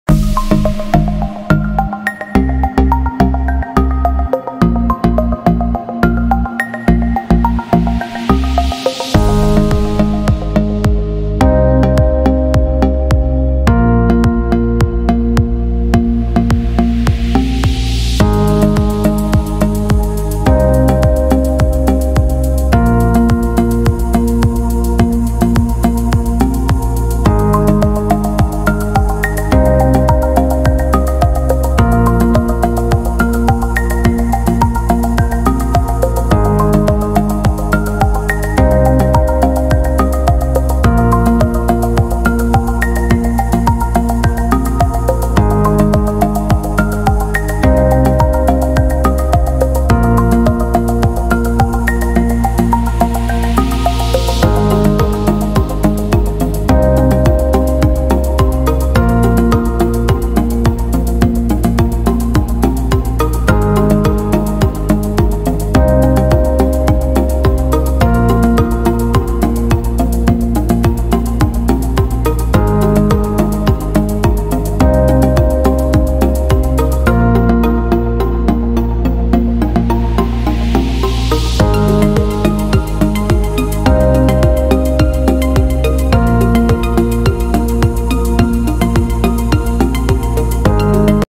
Border Collie Pastel Dog Portrait
A pastel portrait of a Border Collie dog that I commissioned for a customer. I hope you enjoy watching the video.